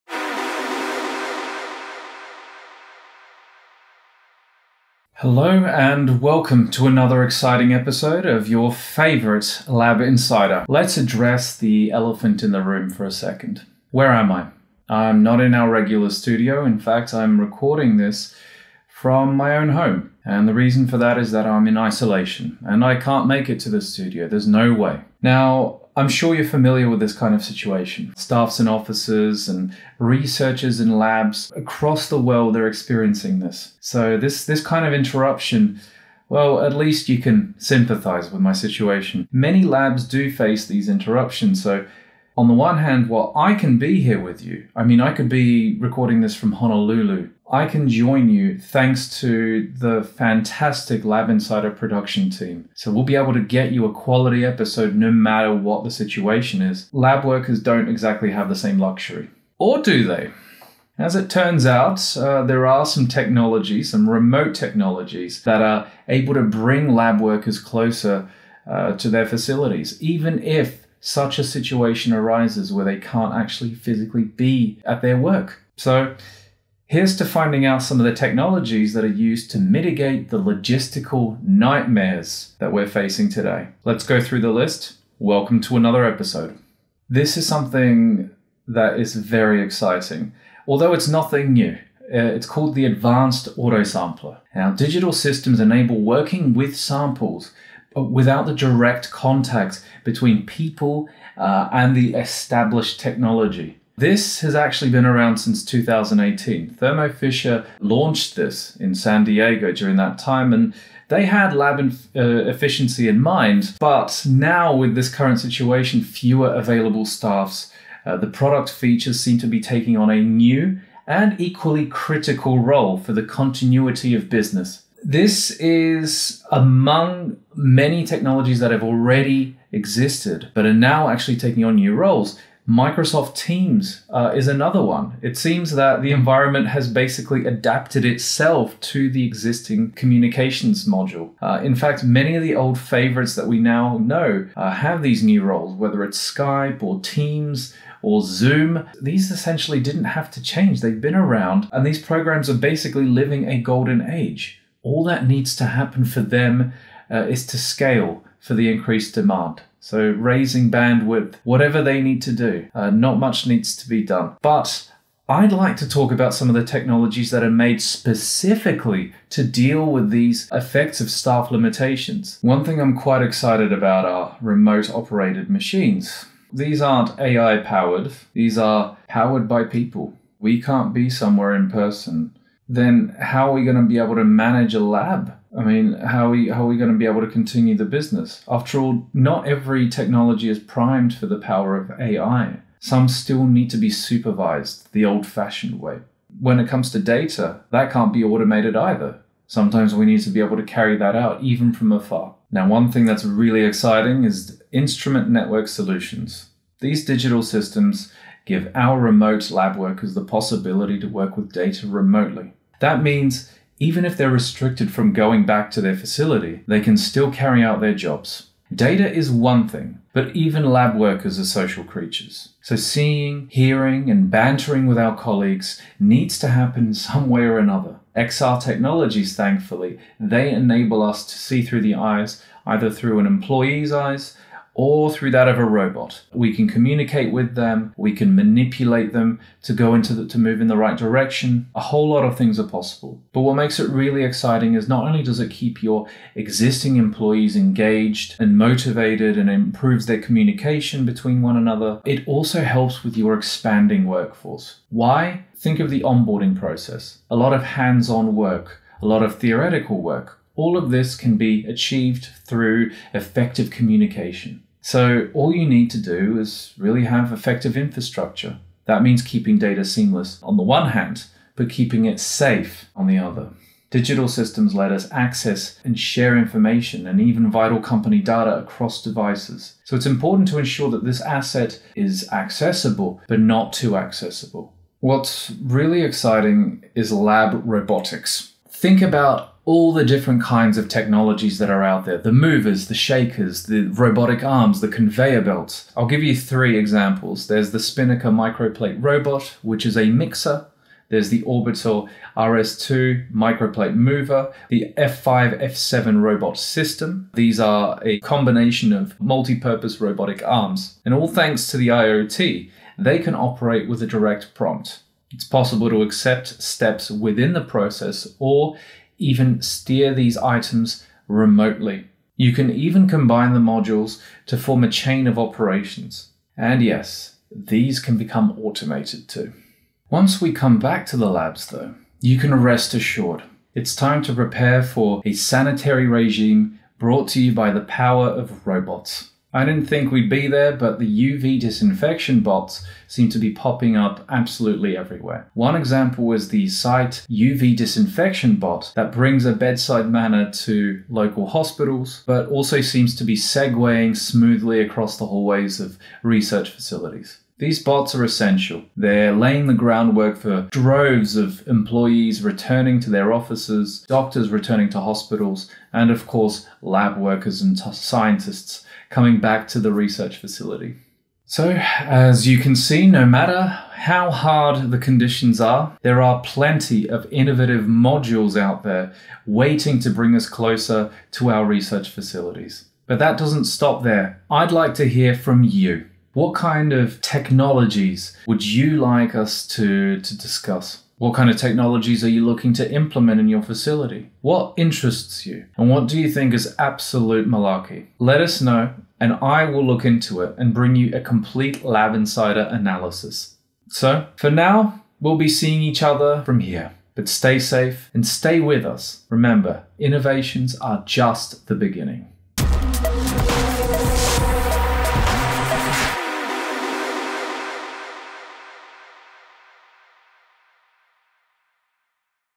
Is Your Lab Quarantine Proof? Explore these Breakthrough Technologies | Lab Insider Special
Staff isolation is manageable for almost every business on the planet. Lab Insider understands, as our host, Max, broadcasts direct from quarantine.

But that’s a situation that could spell disaster for laboratories, where hands-on working environments often remain the norm. We understand this, and as a tribute to lab and research staff facing this problem around the world, our episode will uncover the most prolific digital solutions enabling labs the power of continuity and production value!

Our episode shows how many of us are adapting – just like many organizations. Through the virtues of technology, laboratories can thrive amid the challenges of this all-too-common situation.

Lab Insider’s isolation episode will demonstrate and embody these solutions. Tune in for more!